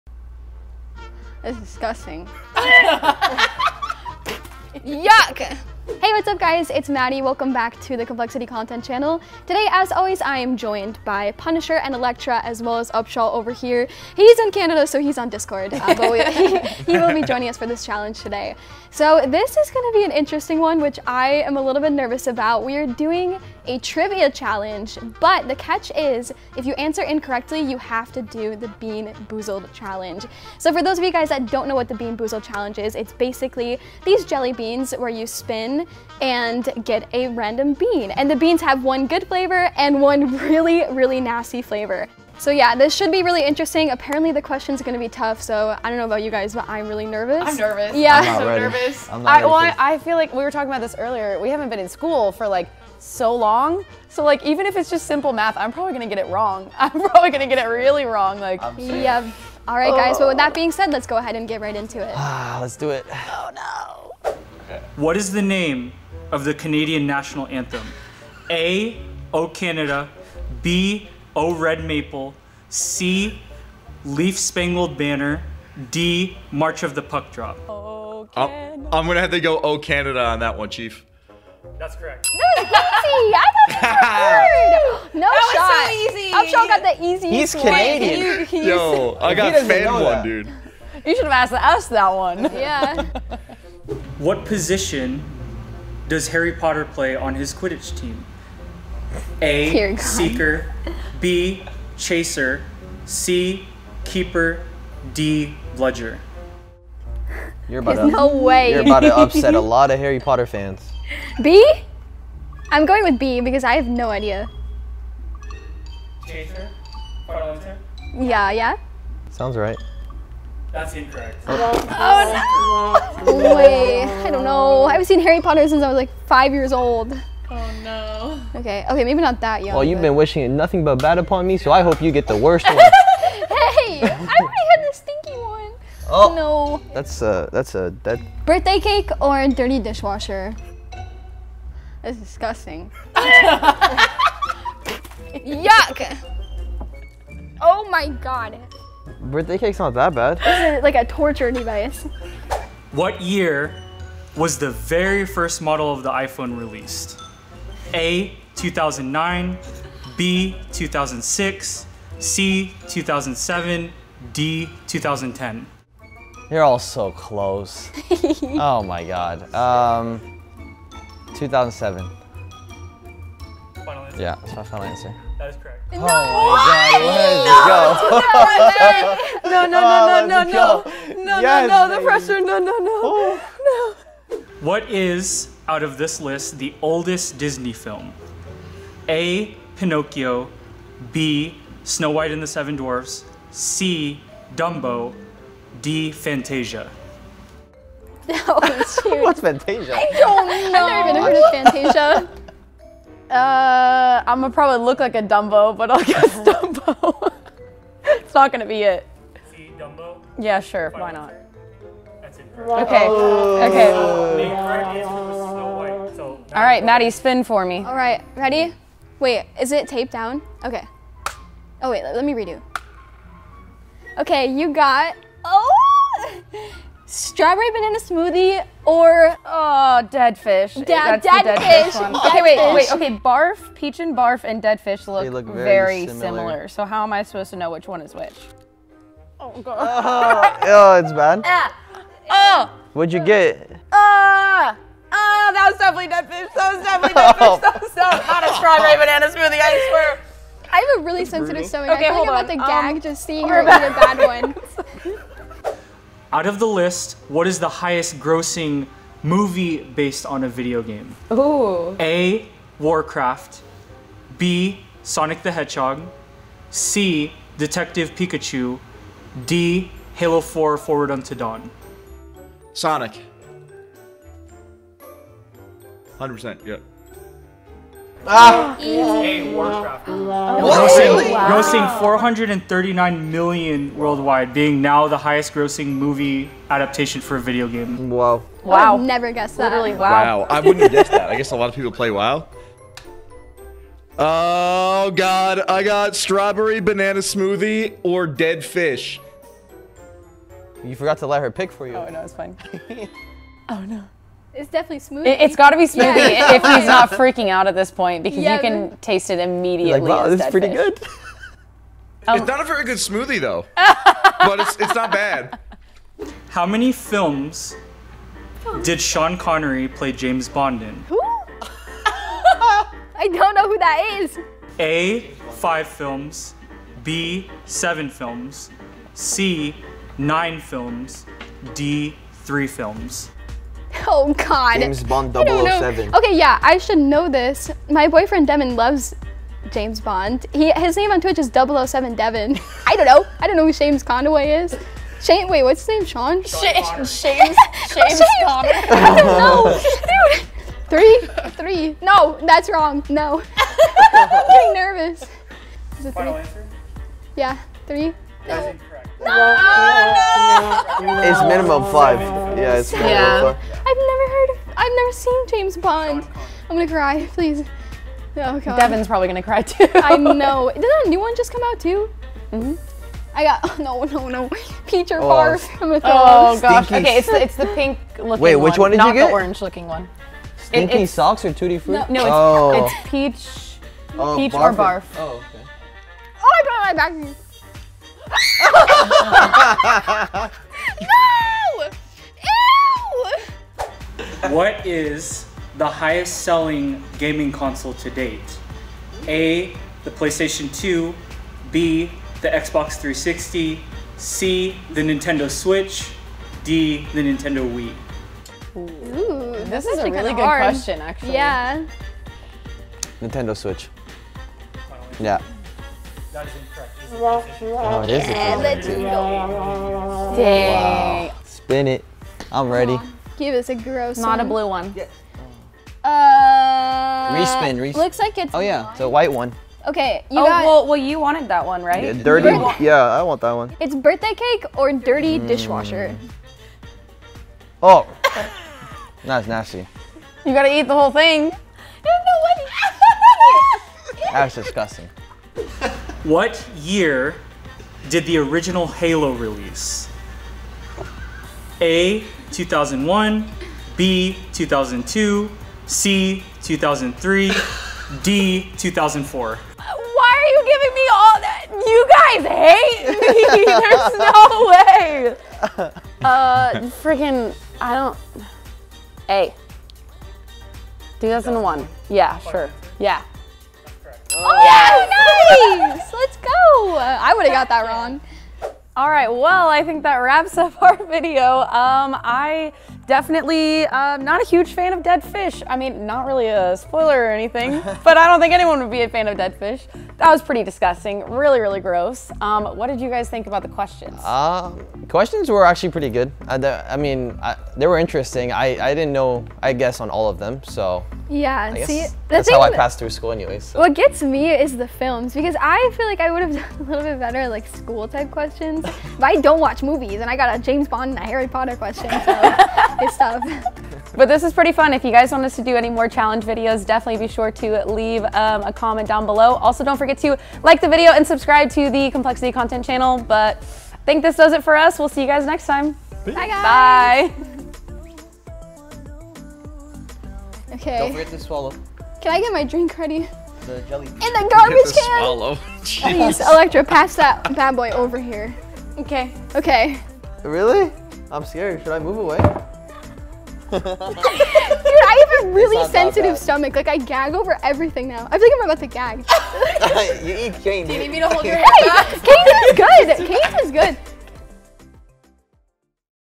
BEAN BOOZLED Trivia Challenge! | Maddynf, Electra, Punisher & Upshall
Answer the question correctly or pay the ultimate price. Join Maddynf, Electra, Punisher, and Upshall in this Bean Boozled trivia challenge. Every wrong answer is a chance for a a delightfully tasty or dreadfully disgusting jelly bean.